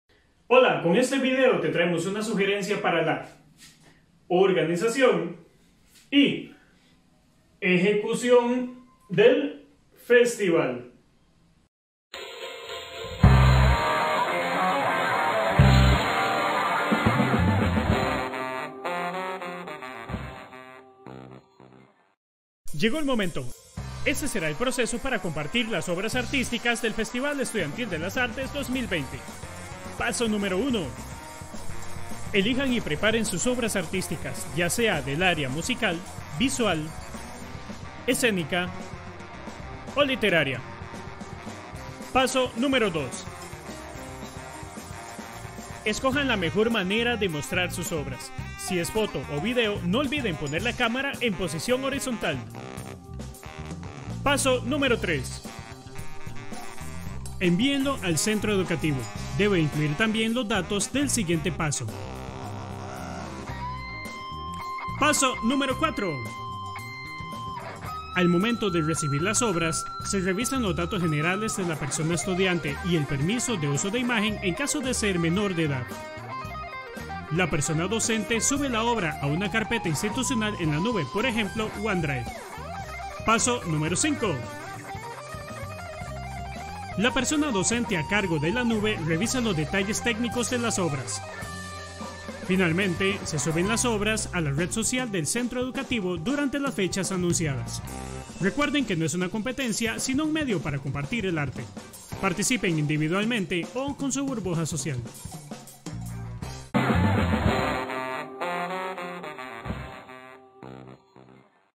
Organizar el FEA 2020: guía rápida para estudiantes y docentes
Les presentamos una guía rápida de cómo organizarse para ejecutar el Festival Estudiantil de las Artes 2020.
Es una guía tanto para estudiantes y docentes, de manera que establezcan los canales de comunicación necesarios para efectuar el FEA.